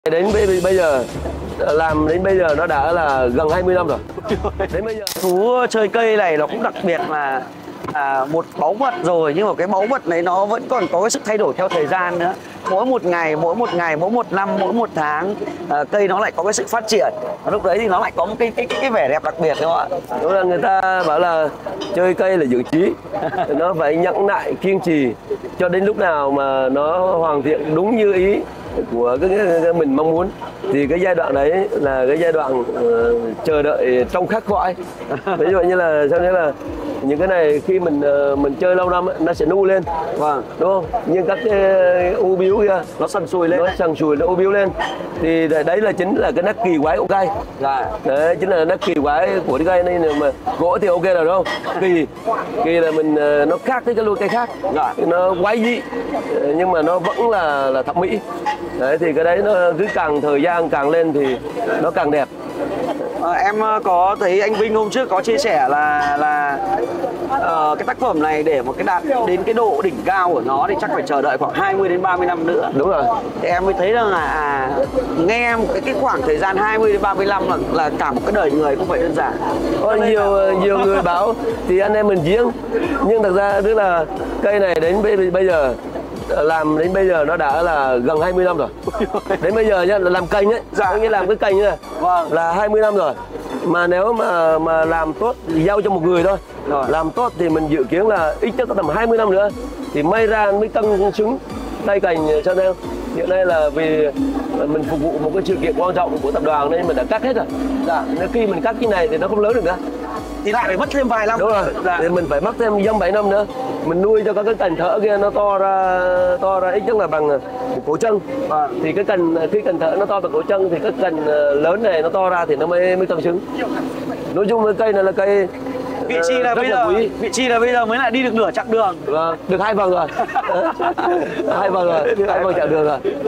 Tận Mắt Chiêm Ngưỡng Cây Thiên Địa Nhân Khổng Lồ: Vẻ Đẹp Kỳ Diệu Của Thiên Nhiên Và Nghệ Thuật
Làm gỗ cũng nhiều năm mà nay em Sơn được chứng kiến tận mắt 1 cây Thiên Địa nhân cực kỳ khủng như thế này. Mời các bác cùng khám phá với em Sơn

🚩 Địa Chỉ: 125A Làng Nghề Thiết Úng - Vân Hà - Đông Anh - Hà Nội